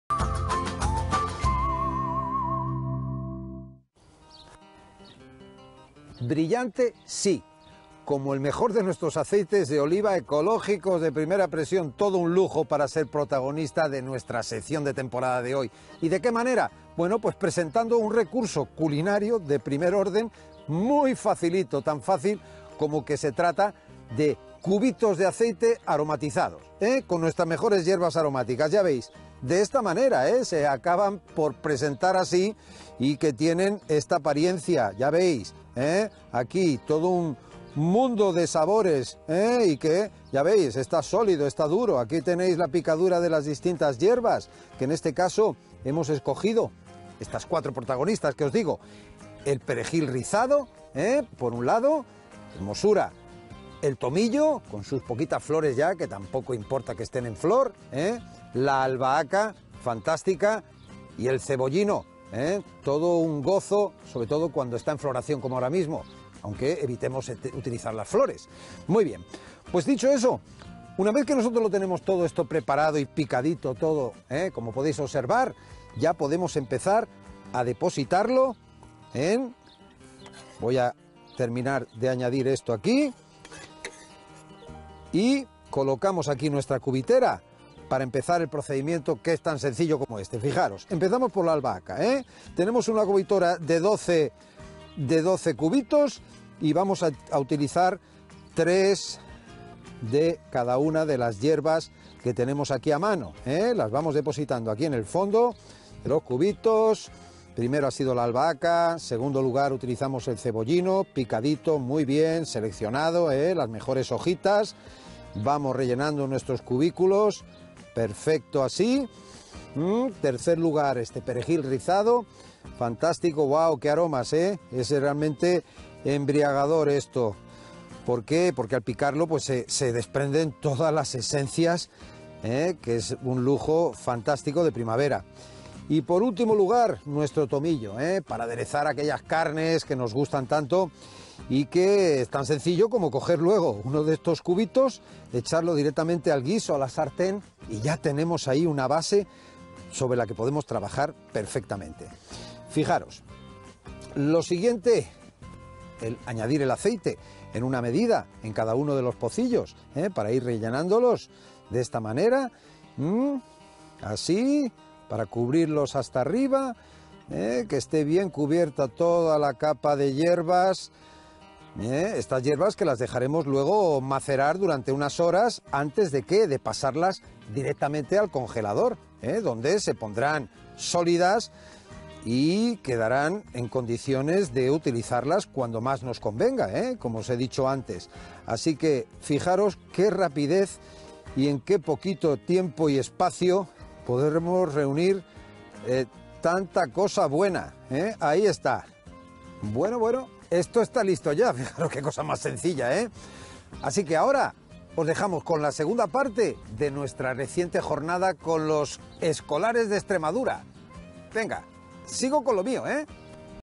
Cómo hacer cubitos de aceite aromatizados
Fernando, en nuestra sección de “Temporada”, nos enseña un curioso recurso culinario: los cubitos de aceite aromatizados, aprenderemos a realizarlos de forma sencilla y amena con perejil rizado, tomillo, albahaca y cebollino.